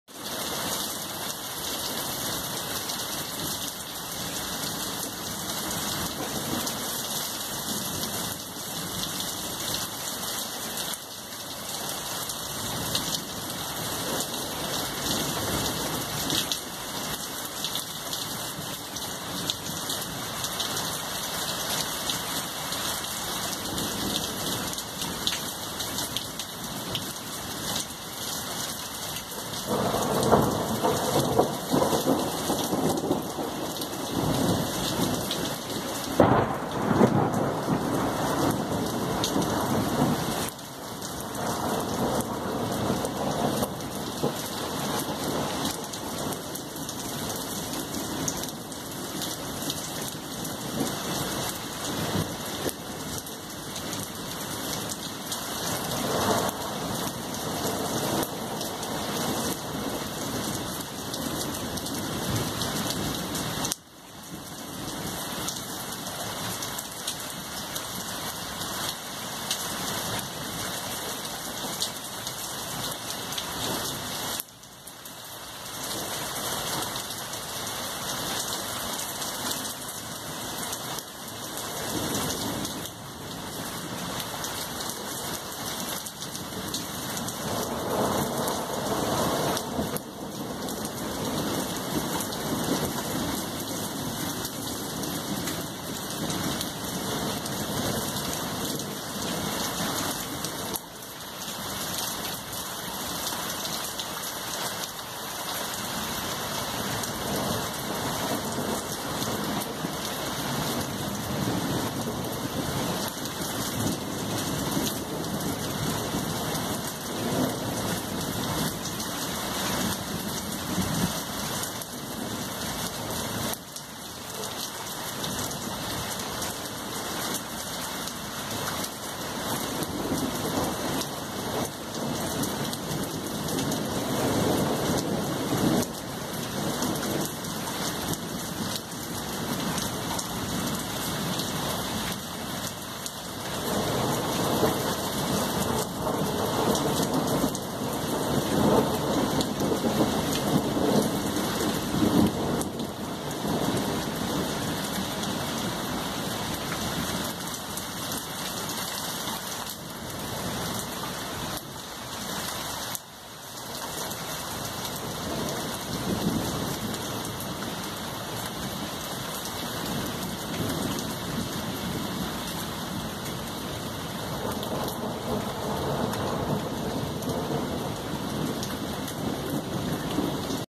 Gewitter über Oldenburg 20190619
Am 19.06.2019 hat es ein wenig gewittert über Oldenburg. Leider viel zu kurz. Aufgenommen mit einem Moto G5.